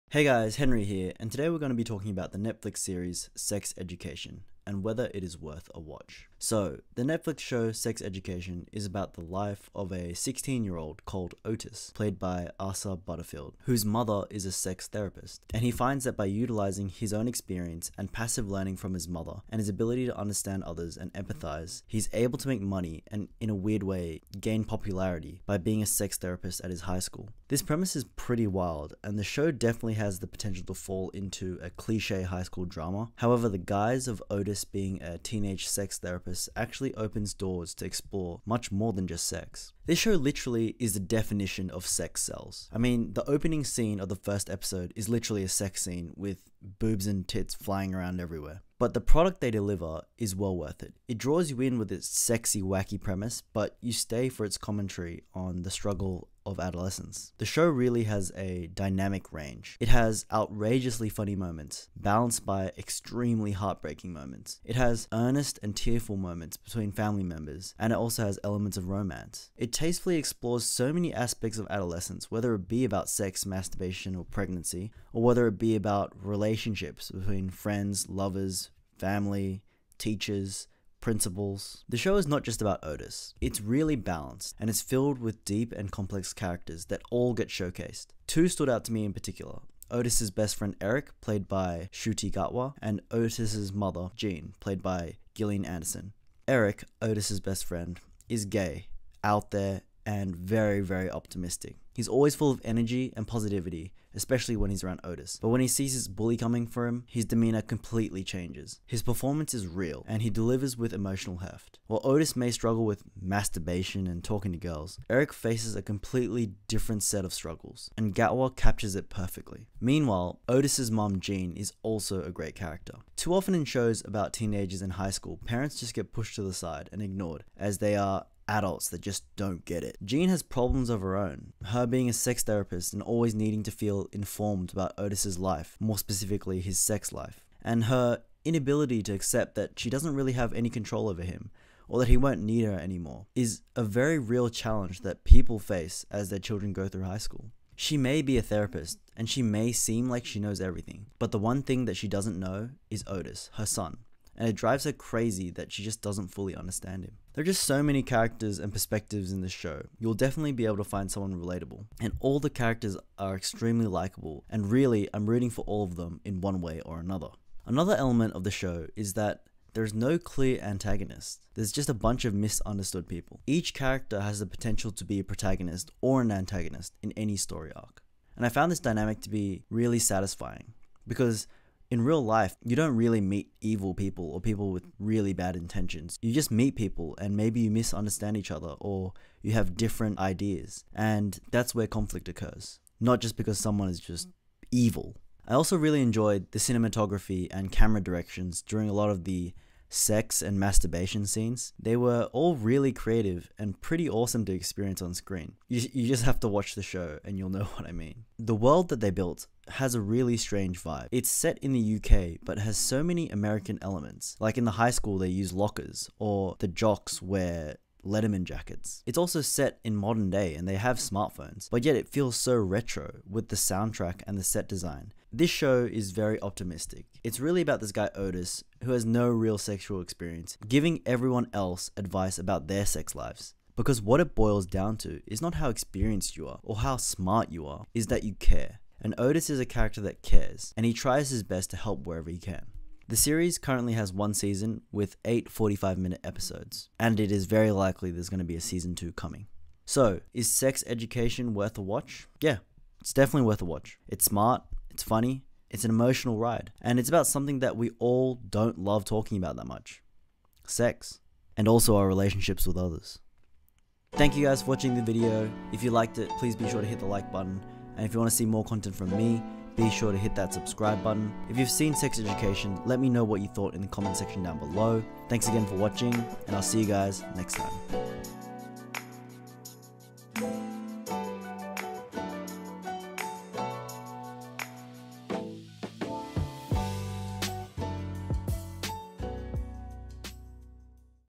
Sex Education Season 1 (Netflix) - Worth a Watch? | TV Show Review
My thoughts on tv series starring Asa Butterfield, Ncuti Gatwa and Gillian Anderson and whether I thought it was worth a watch.

Netflix's Sex Education has 8 episodes in season 1. It is a British comedy drama following the life of Otis, who has a sex therapist for a mother, and decides to become a sex therapist at his high school.